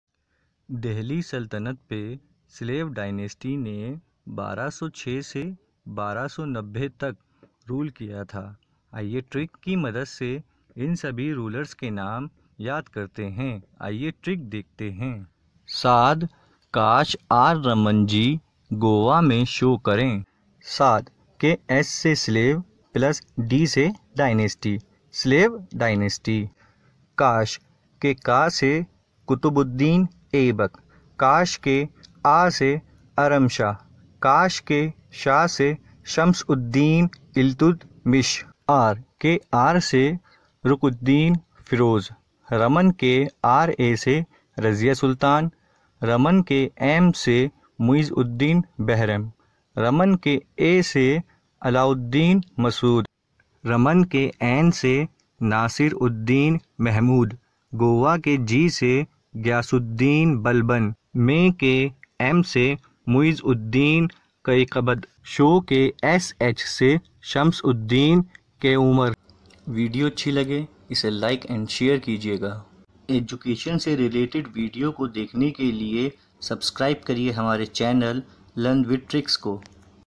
Slave Dynasty [1206-1290] ( स्लेव डायनेस्टी के सभी राजाओं के नाम ट्रक से याद करें)
In this video you will learn about Slave Dynasty { SSC, UPSC, CTET, and other government exam. }
Recall all Slave Dynasty kings by Trick.
स्लेव डायनेस्टी के सभी राजाओं के नाम ट्रिक कि मदद से याद करें!